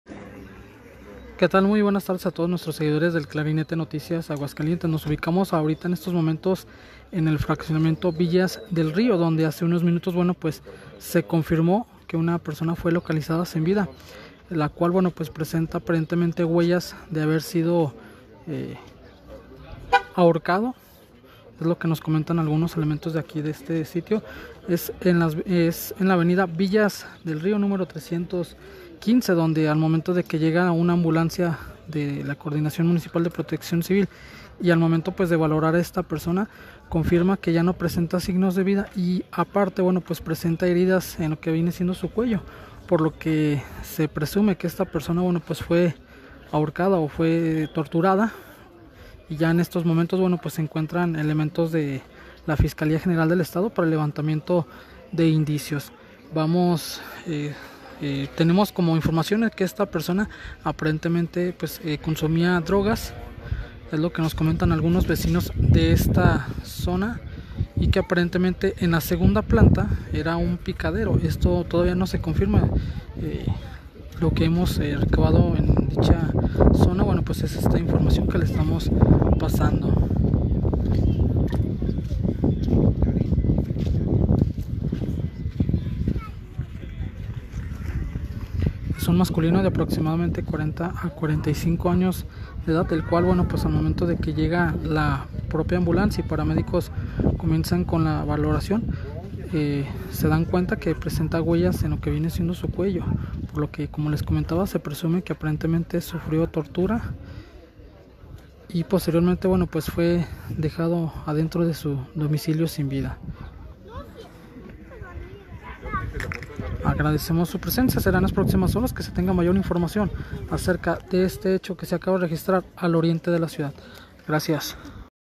Adicto al cristal fallece frente a su esposa en Villas del Rio - Aguascalientes: Posible Homicidio
• POLICÍAS MUNICIPALES DE AGUASCALIENTES ATIENDE REPORTE DE PERSONA SIN VIDA EN EL INTERIOR DE UN DOMICILIO DEL FRACCIONAMIENTO VILLAS DEL RIO.

• PERSONAL DE LA DIRECCIÓN GENERAL DE INVESTIGACIÓN PERICIAL DETERMINARÁN LAS CAUSAS DE LA MUERTE.

Elementos de la Policía Municipal de Aguascalientes atendieron el reporte de persona inconsciente en el interior de un domicilio del fraccionamiento Villas del Río, tras el arribo de los oficiales se confirmó por paramédicos que efectivamente había una persona de sexo masculino sin vida, por lo que al sitio arribó personal de la Fiscalía General del Estado.

El reporte se recibió a las 11:30 horas de este miércoles en el C4 Municipal de Aguascalientes, en el que se manifestaba que en la casa marcada con el número 314 interior C de la Calle Villas del Río, en el fraccionamiento Villas del Río, se encontraba una persona al parecer sin vida, motivo por el que de inmediato se trasladaron elementos de la policía preventiva del Destacamento Terán Norte así como paramédicos.

A su arribo, se confirmó por parte de paramédicos de la Coordinación Municipal de Protección Civil la ausencia de signos vitales de quien en vida respondía al nombre de Jorge N de 36 años de edad, a decir de su pareja sentimental continuamente tenían discusiones y problemas familiares, por lo que el día de hoy al estar discutiendo, el hoy occiso se desvaneció frente a ella quedando inconsciente en una de las habitaciones, a pesar de haberle intentado reanimar con RCP no logró que respirara, manifestando que Jorge N tenía antecedentes de adicción al cristal.

Finalmente, al lugar del reporte arribo personal de la Dirección General de Investigación Pericial, quienes se encargarían del levantamiento de indicios y el posterior traslado del cuerpo al Servicio Médico Forense para practicarle la necropsia de ley y determinar las causas de la muerte.